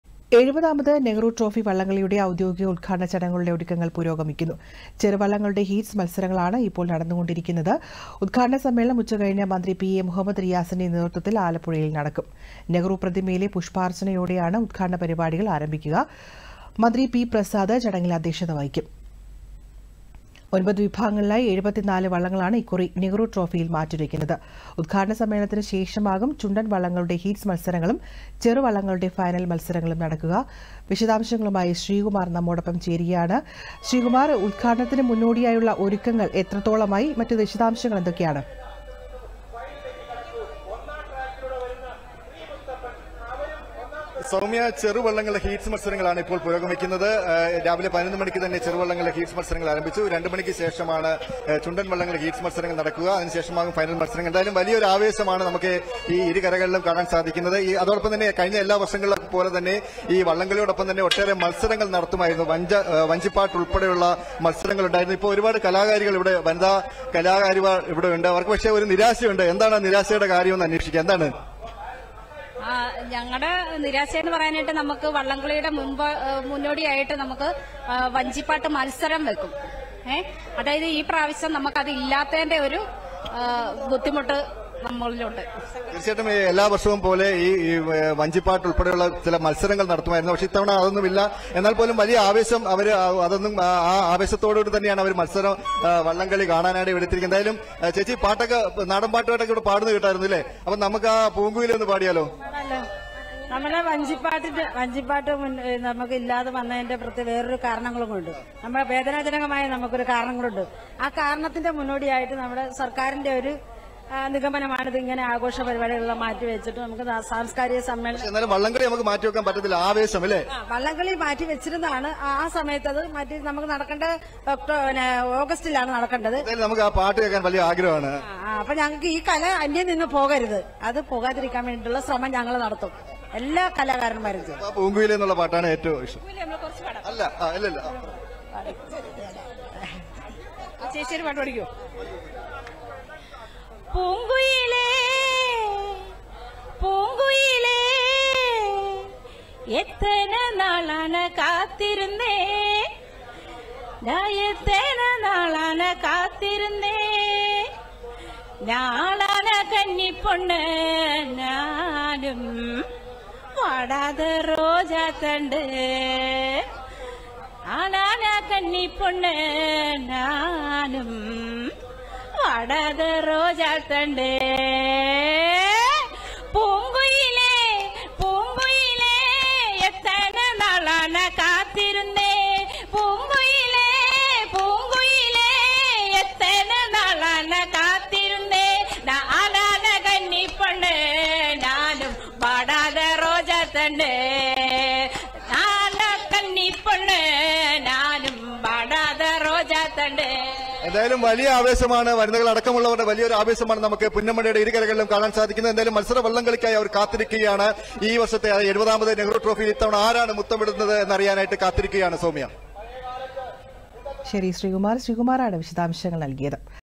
ഇത്തവണ വഞ്ചിപ്പാട് മത്സരമില്ലെങ്കിലും ആർത്തുപാടി ആവേശത്തിലാണ് പുന്നമട
70-ാമത് നെഹ്റു ട്രോഫി വള്ളംകളിയുടെ ഔദ്യോഗിക ഉദ്ഘാടന ചടങ്ങുകളുടെ ഒരുക്കങ്ങൾ പുരോഗമിക്കുന്നു.

ചെറുവള്ളങ്ങളുടെ ഹീറ്റ്സ് മത്സരങ്ങളാണ് ഇപ്പോൾ നടന്നുകൊണ്ടിരിക്കുന്നത്.

ഉദ്ഘാടന സമ്മേളനം ഉച്ചകഴിഞ്ഞ് മന്ത്രി പി എ മുഹമ്മദ് റിയാസിന്റെ നേതൃത്വത്തിൽ ആലപ്പുഴയിൽ നടക്കും.

നെഹ്റു പ്രതിമയിലെ പുഷ്പ്പാര്‍ച്ചനയോടെയാണ് ഉദ്ഘാടന പരിപാടികള്‍ ആരംഭിക്കുക.

മന്ത്രി പി.പ്രസാദ് ചടങ്ങിൽ അധ്യക്ഷത വഹിക്കും.

ഒന്‍പത് വിഭാഗങ്ങളിലായി 74 വള്ളങ്ങളാണ് ഇക്കുറി നെഹ്‌റു ട്രോഫിയില്‍ മാറ്റുരയ്ക്കുന്നത്.

ഉദ്ഘാടന സമ്മേളനത്തിനു ശേഷമാകും ചുണ്ടന്‍ വള്ളങ്ങളുടെ ഹീറ്റ്സ് മത്സരങ്ങളും ചെറു വള്ളങ്ങളുടെ ഫൈനല്‍ മത്സരങ്ങളും നടക്കുക.